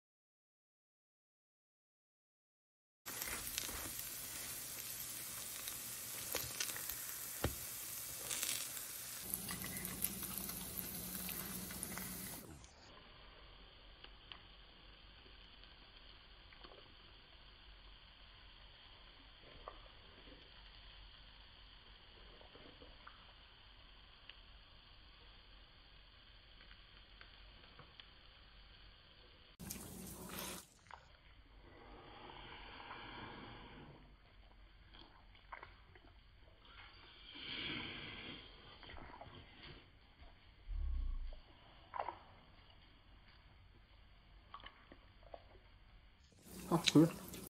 快手萝卜丝饼
半个白萝卜切丝，用盐腌15分钟去一下涩味，沥干。
脆皮面糊比例：
面粉100克、
生粉20克
色拉油10ml
泡打粉5克
清水120ml
其他调味料根据口味加。

煎的时候要多放油，无油不脆。先放一层糊，过几秒放萝卜丝，底下结壳了再放一层糊，然后翻面，油不够了要及时加点。出锅后吸一下油。